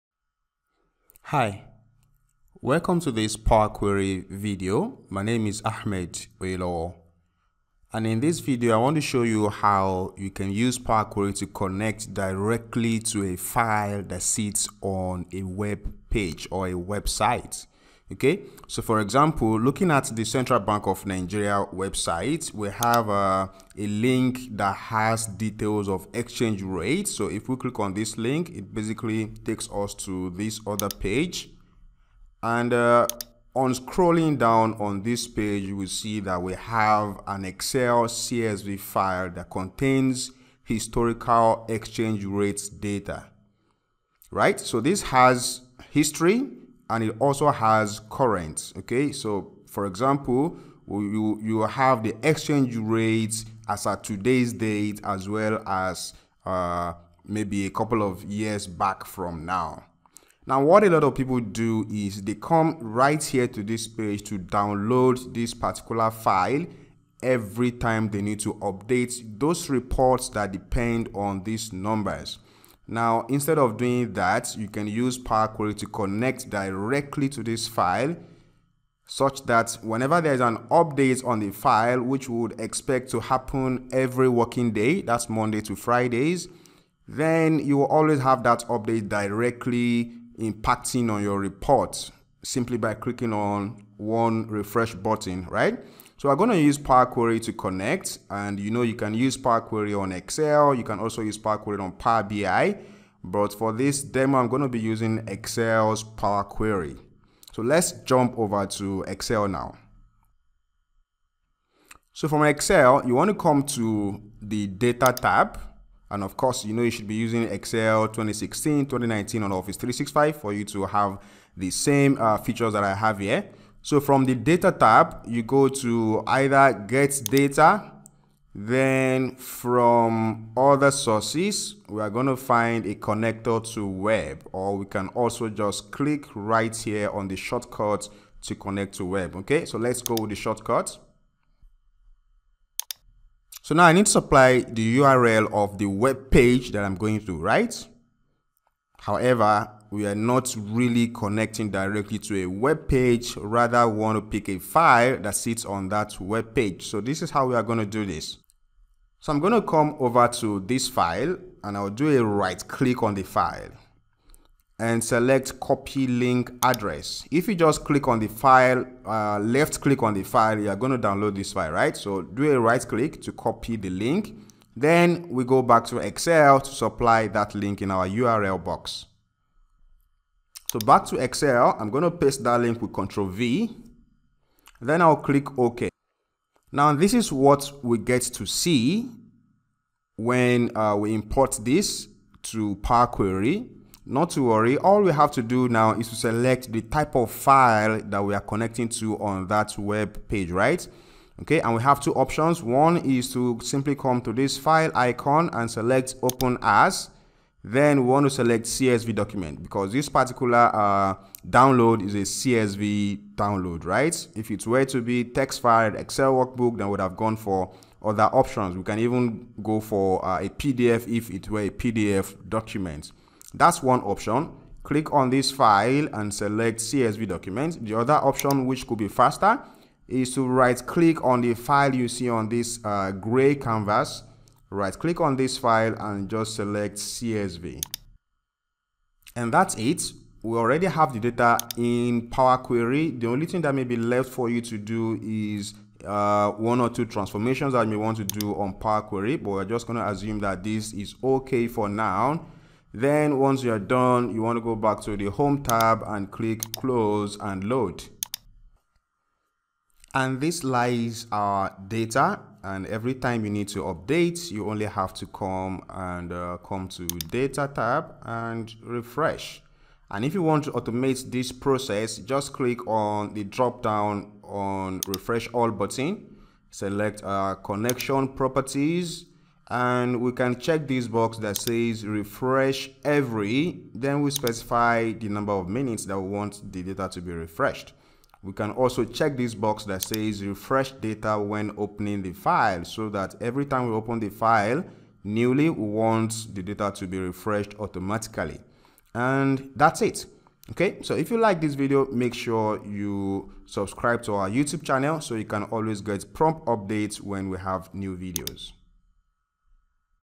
How to connect Excel to a file on the web
This video shows how you can use Power Query to connect directly to a file sitting on a web page instead of downloading the file. You will also see how to automate the refresh for that data once loaded to excel.

This demo used the Central Bank of Nigeria (CBN) Exchange Rate Web Page with the following

In the video, we connected to CBN Exchange Rates directly from Excel.